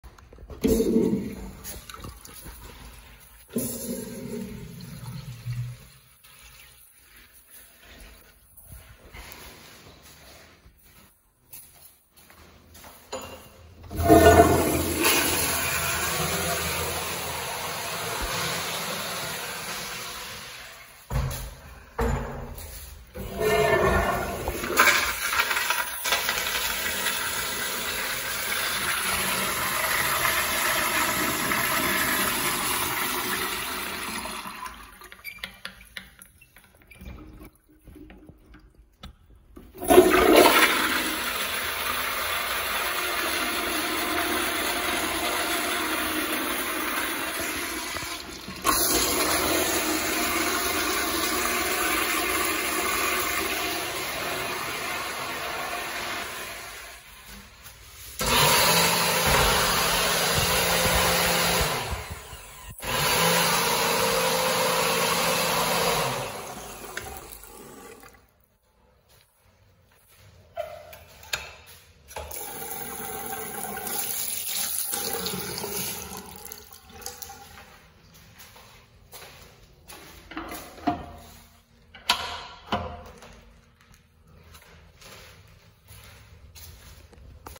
Jones Beach West End Boat Basin Men’s Restroom
This restroom is barely open!! I was there last time I filmed at jones beach and it was locked. Rare Toto urinals!! The toilets are not my favorite. Older building. The women’s room has 3 Toto CT708’s and a new afwall which I didn’t bother as this lot was empty.